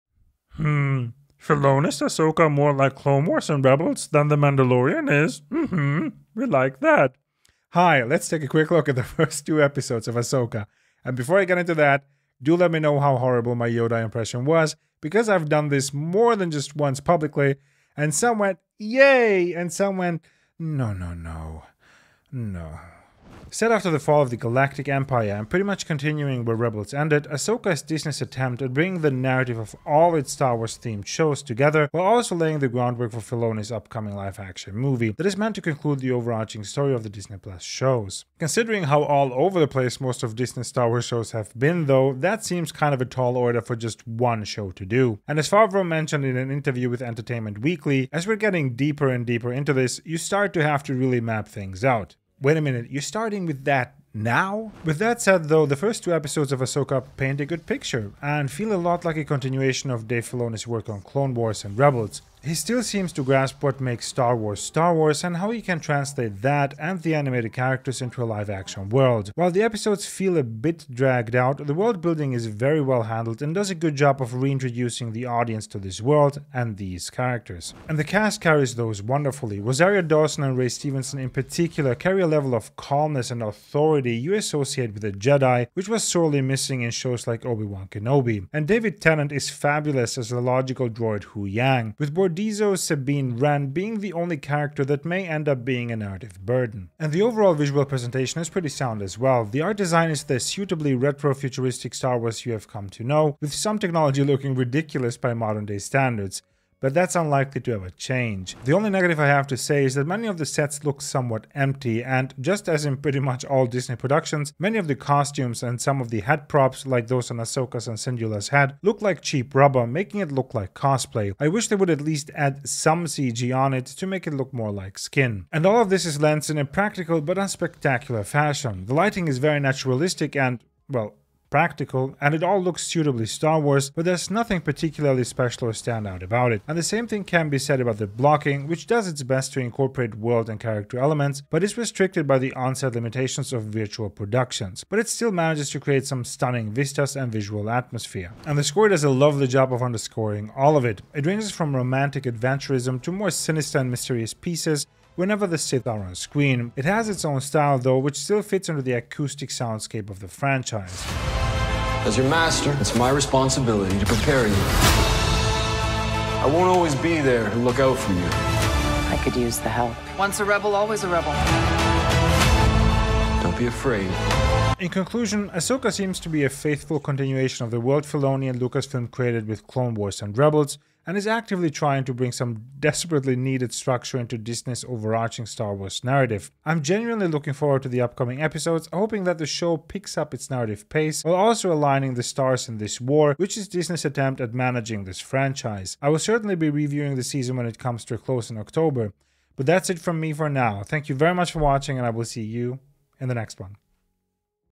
AHSOKA Episode 1 & 2 Review - More Star Wars Rebels than Mandalorian
A spoiler- & politics-free review and reaction of Dave Filoni's Star Wars Original series Ahsoka.

Dave Filoni
George Lucas
Rosario Dawson ...  Ahsoka Tano
Natasha Liu Bordizzo ...  Sabine Wren
Wes Chatham ...  Captain Enoch
Mary Elizabeth Winstead ...  Hera Syndulla
Ray Stevenson ...  Baylan Skoll
Ivanna Sakhno ...  Shin Hati
Diana Lee Inosanto ...  Morgan Elsbeth
David Tennant ...  Huyang
Eman Esfandi ...  Ezra Bridger
Mark Rolston ...  Captain Hayle
Peter Jacobson ...  Myn Weaver
Shelby Young ...  C1-D11
Shakira Barrera ...  First Officer Jensen Corbyt
Clancy Brown ...  Governor Ryder Azadi
Lucasfilm
Disney+
Disney

After the fall of the Galactic Empire, former Jedi Knight Ahsoka Tano investigates an emerging threat to a vulnerable galaxy.

Part One: Master and Apprentice
Part Two: Toil and Trouble

Star Wars: Ahsoka
スター・ウォーズ:アソーカ
Асока
亞蘇卡

00:00 Intro
00:23 Story
00:54 Directing, Writing & Editing
01:17 Characters & Performances
01:36 Production Design & Effects
02:05 Cinematography
02:28 Score & Soundscape
02:54 Conclusion